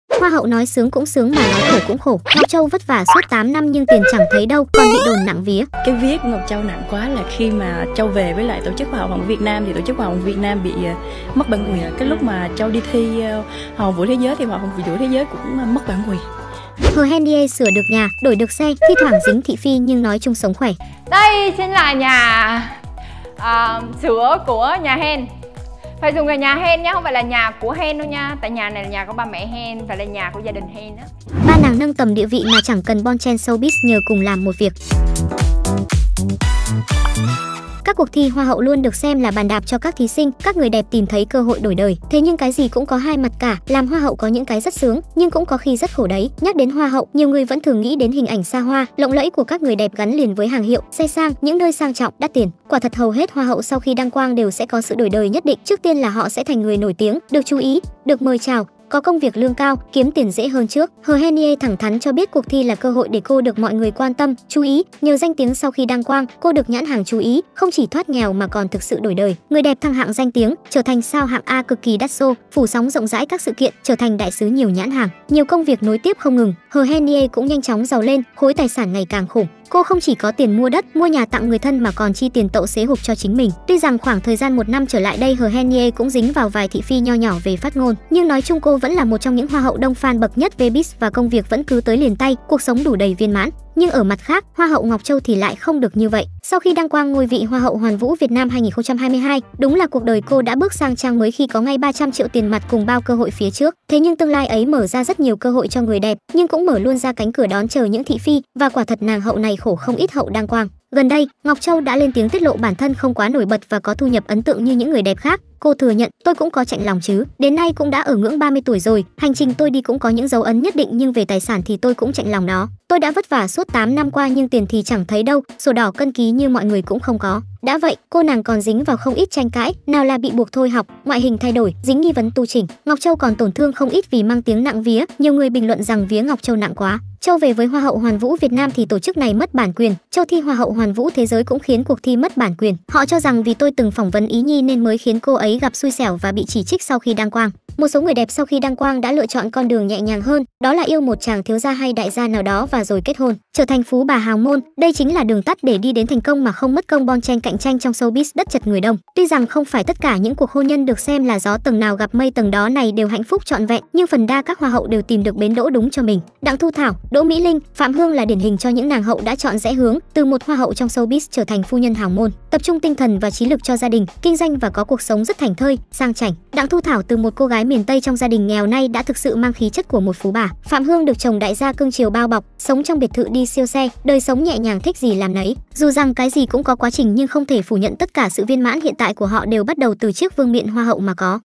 Hoa hậu nói sướng cũng sướng mà nói khổ cũng khổ: Ngọc Châu vất vả suốt 8 năm
Các cuộc thi hoa hậu luôn được xem là bàn đạp cho các thí sinh, các người đẹp tìm thấy cơ hội đổi đời, thế nhưng cái gì cũng có 2 mặt cả, làm hoa hậu có những cái rất sướng, nhưng cũng có khi rất khổ đấy.